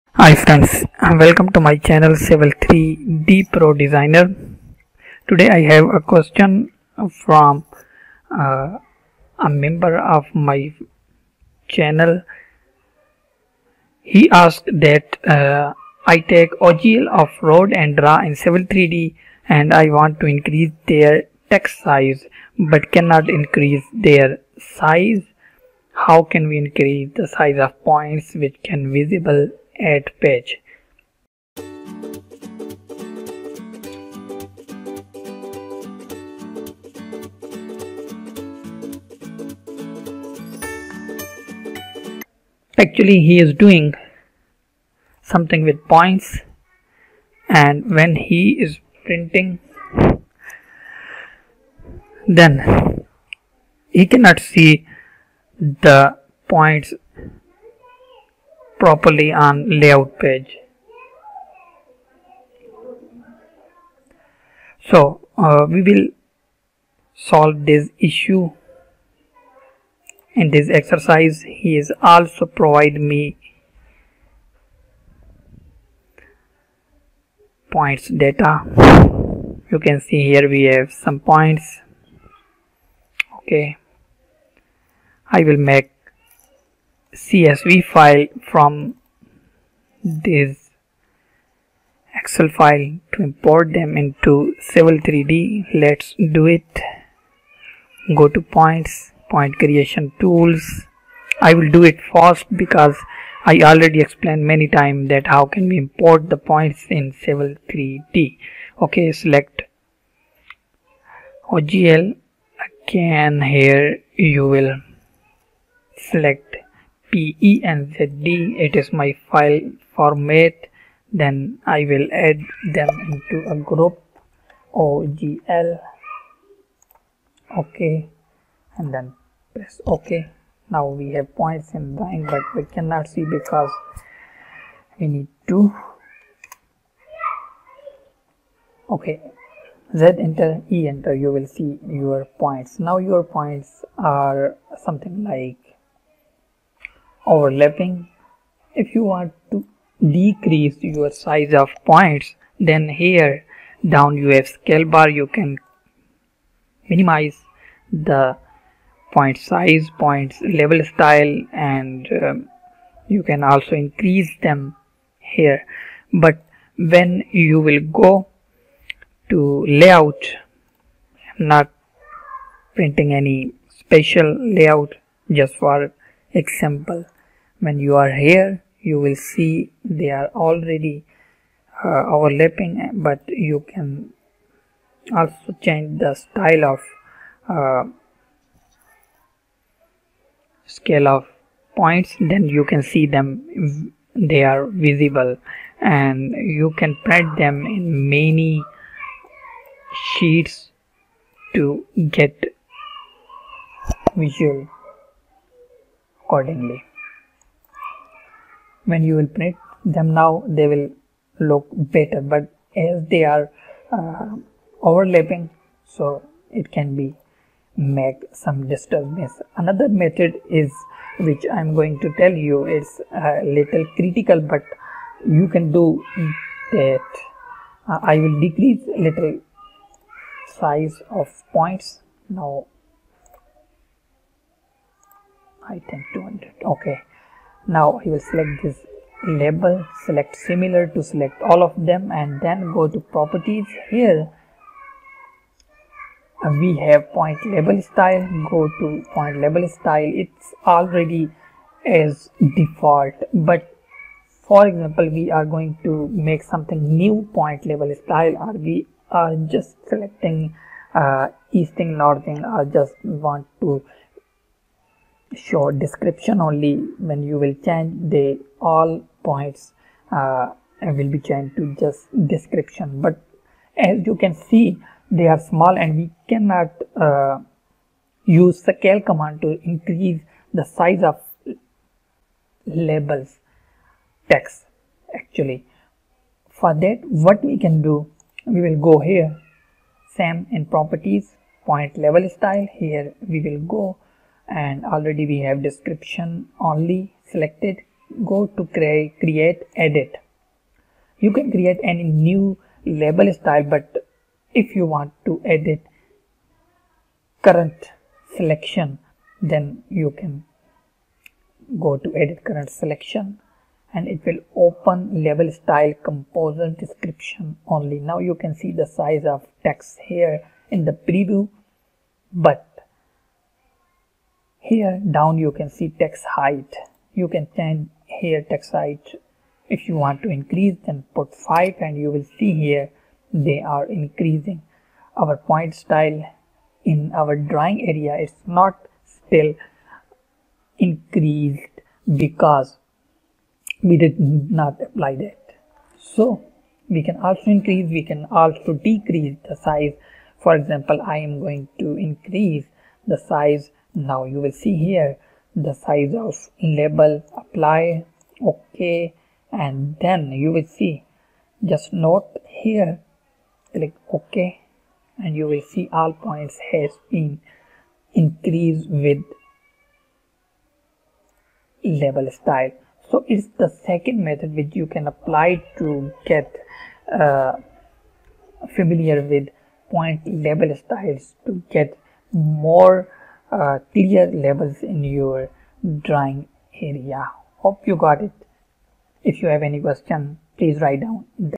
How to Increase and Decrease Point Labels Text Size in Civil 3d|Edit Label Scale in AutoCAD Civil 3d
Welcome to my comprehensive tutorial on how to increase and decrease point labels text size in Civil 3D, the industry-leading software for civil engineering and design. In this video, I'll provide you with a step-by-step guide to help you enhance your proficiency and efficiency in point labeling.

Are your point labels too small or too large, causing readability issues in your Civil 3D projects? Look no further! just follow all steps to control and optimize the text size of your point labels like a pro.